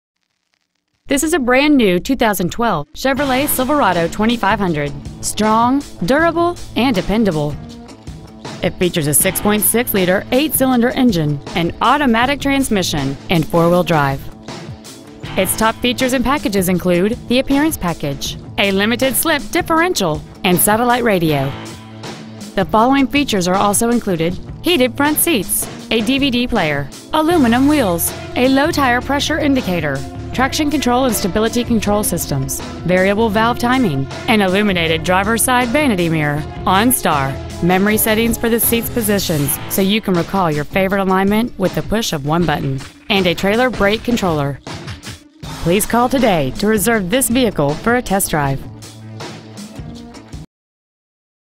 2012 Chevrolet Silverado 2500 Cumming GA 30040
Year : 2012
 Make : Chevrolet
 Model : Silverado 2500
 Engine : Turbocharged Diesel V8 6.6L/403
 Trans . : Automatic
 Exterior : Mocha Steel Metallic
 Miles : 8
 Interior : Dark Cashmere/Light Cashmere
 ANDEAN MOTOR CO
 527 Atlanta Rd
 Cumming , GA 30040
 2012 Chevrolet Silverado 2500 Cumming GA